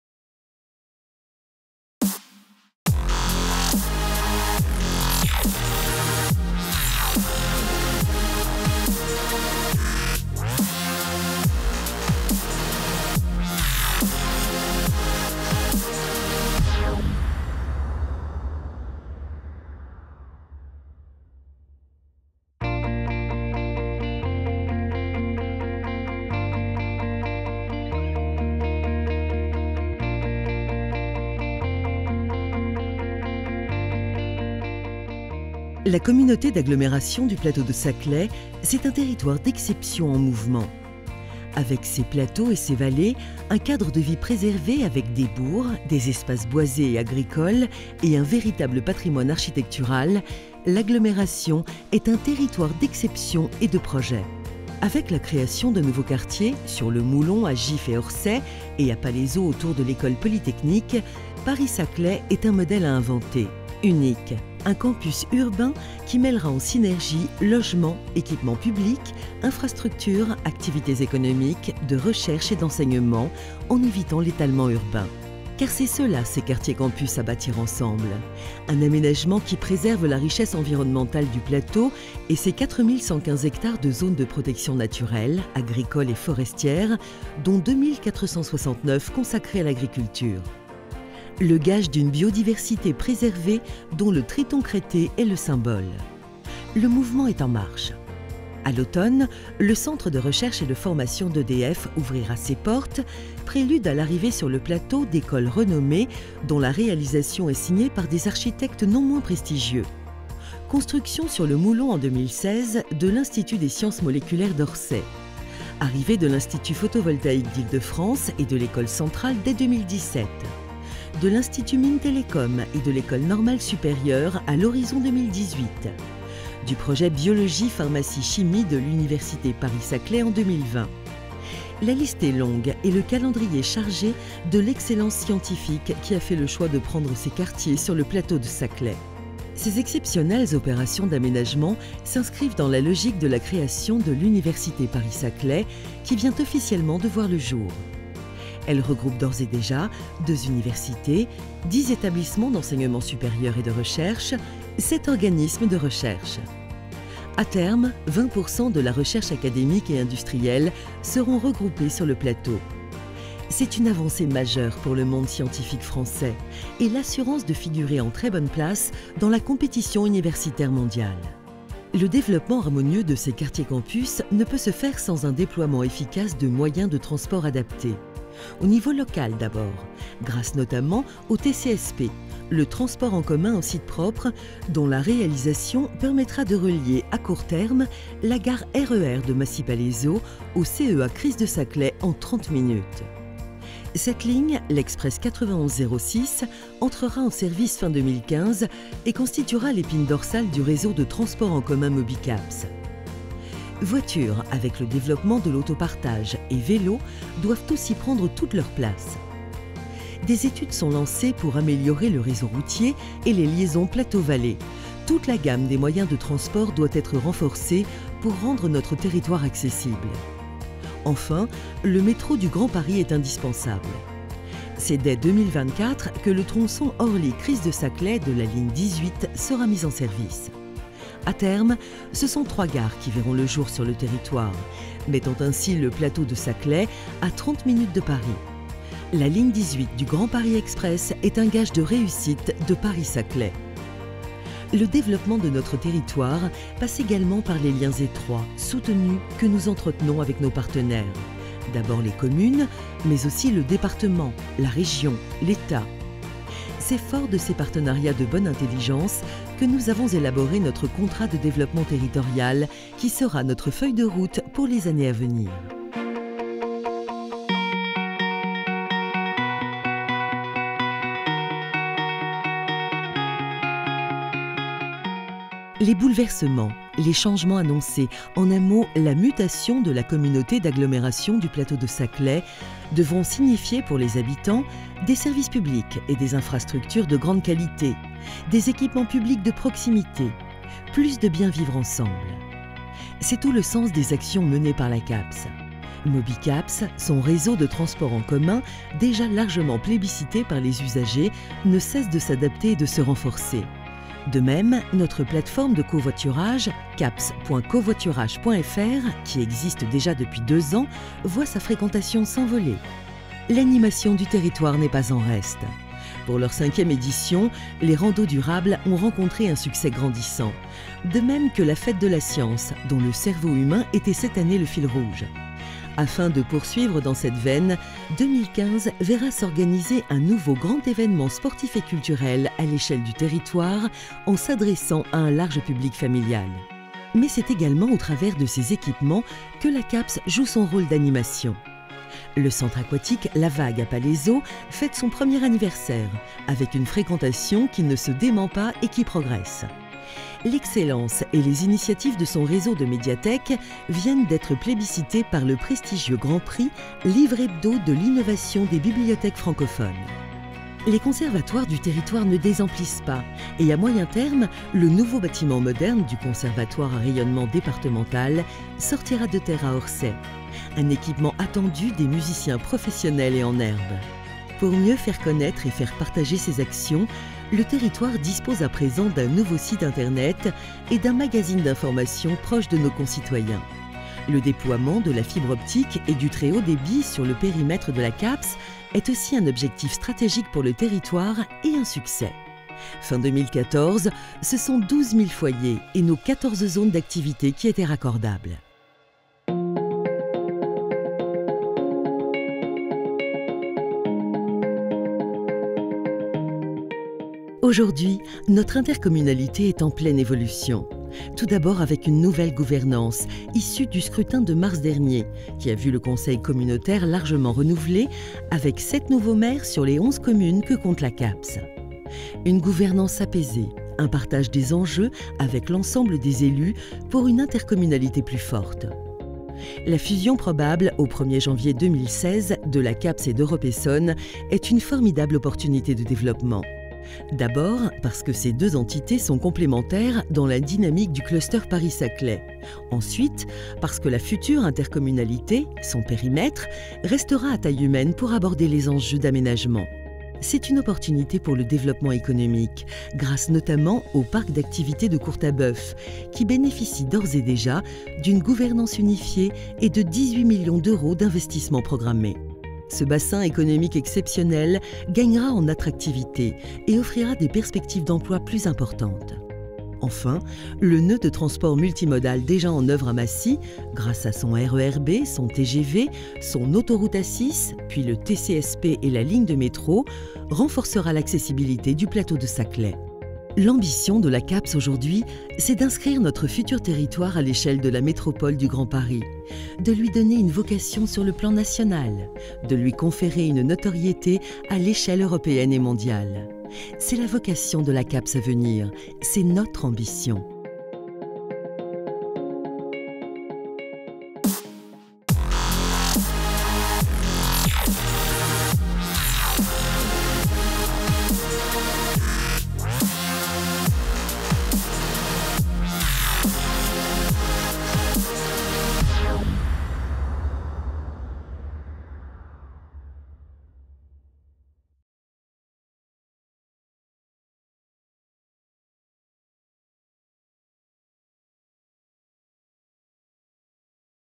Voeux de la CAPS 2015
Les grands événements de l'année 2014, les perspectives et les orientations de l'année 2015, retrouvez ces grandes étapes passées et à venir dans ce film projeté lors de la cérémonie des voeux de la CAPS.